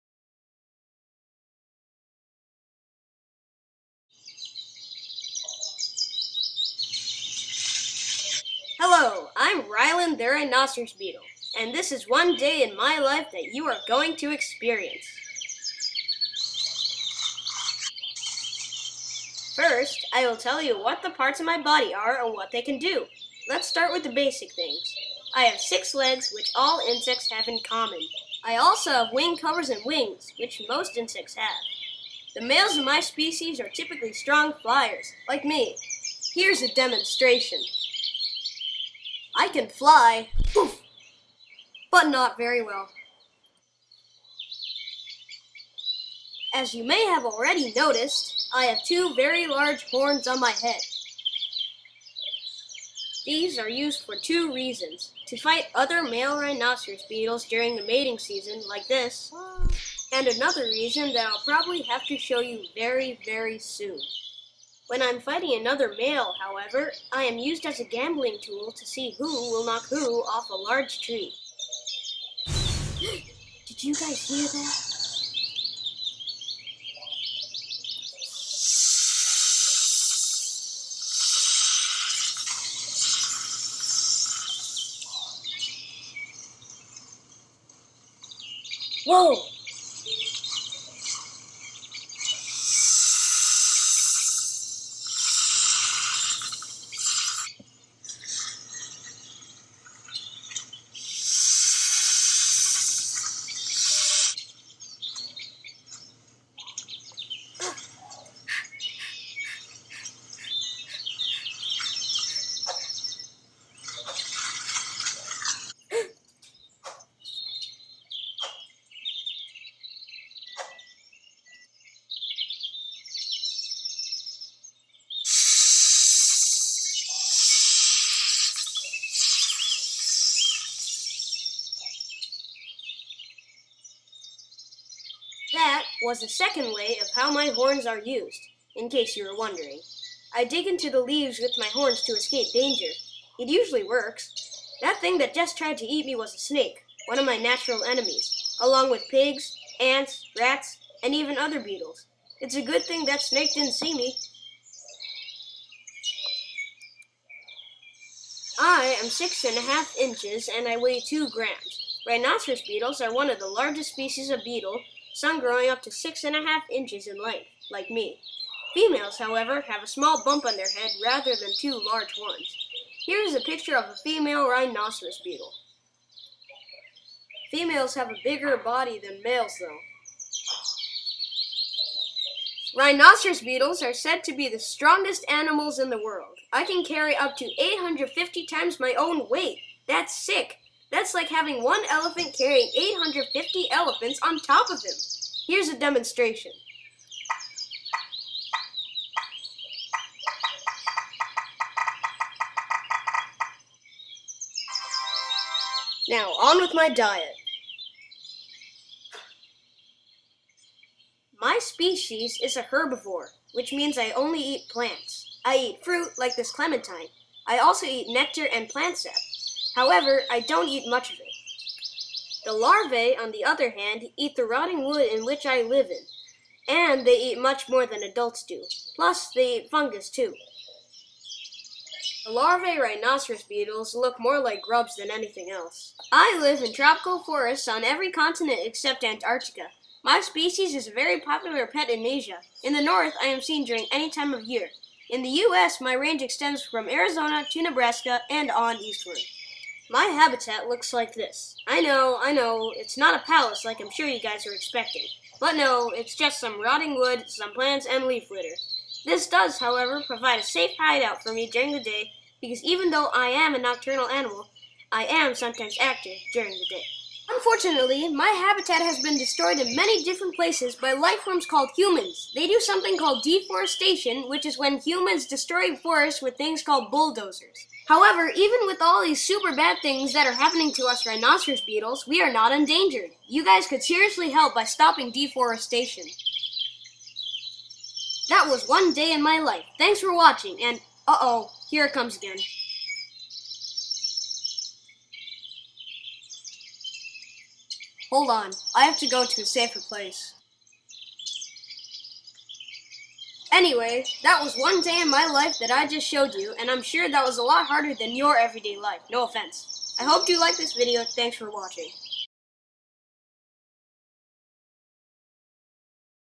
Rhinoceros beetle stop motion
This is my oral presentation for my animal report project in school about the Rhinoceros beetle.